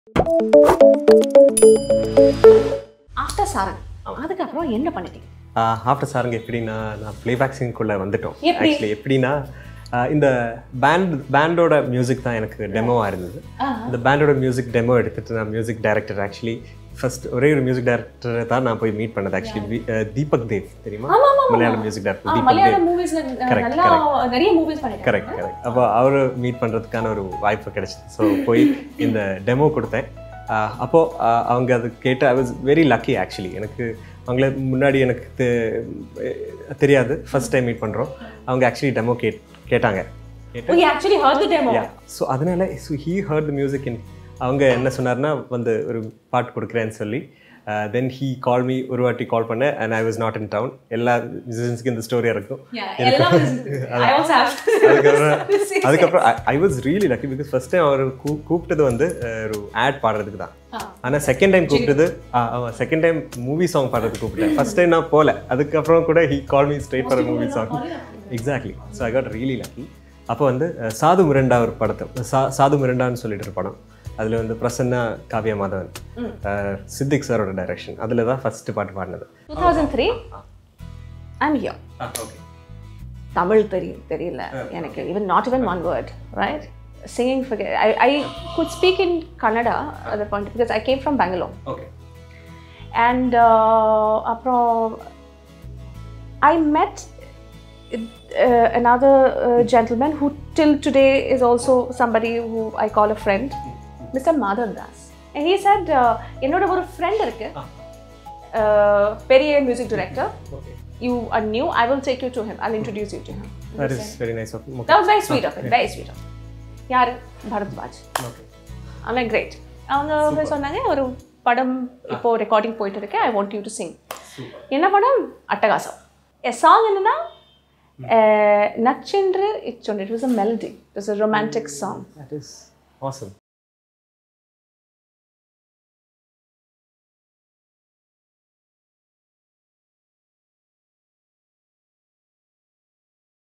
Suchith Suresan and Ujjayinee Roy | Soundproof
Suchith and Ujjayinee share their first recording experiences!

Sound proof is a Fun and engaging interaction between musicians